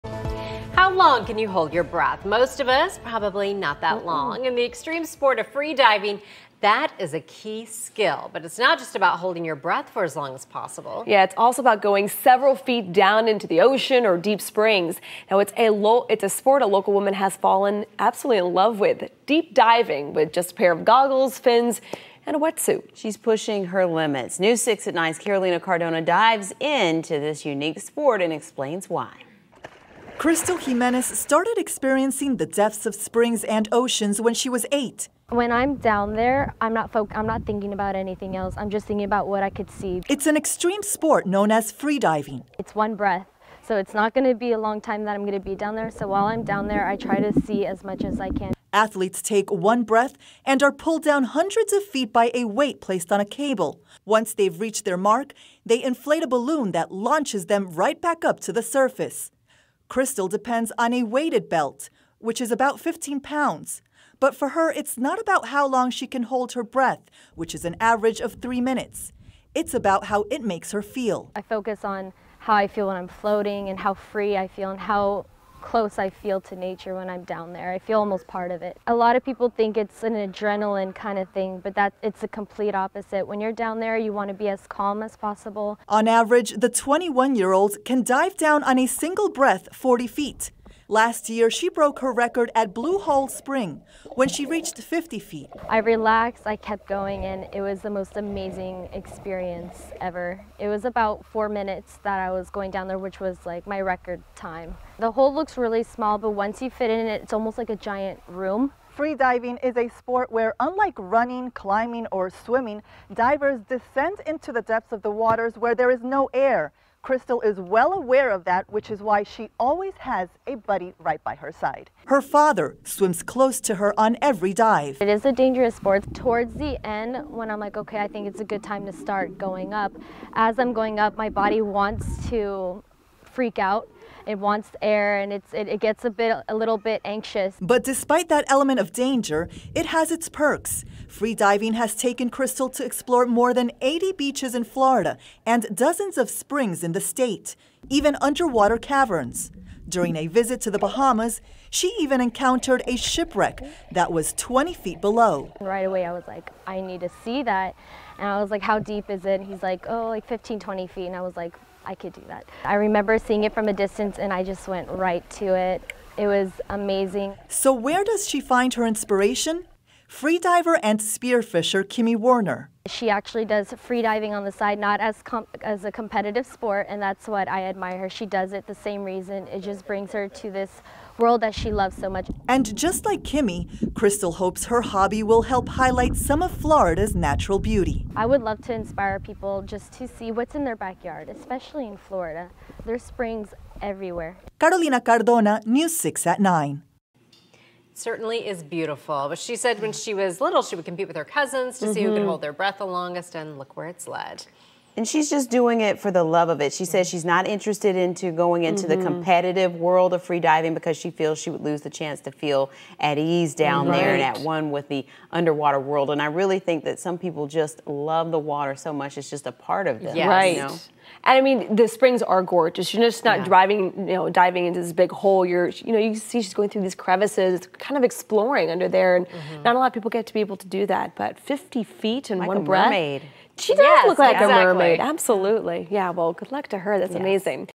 Orlando woman explores Florida's waters through free diving
A local woman free dives in local springs.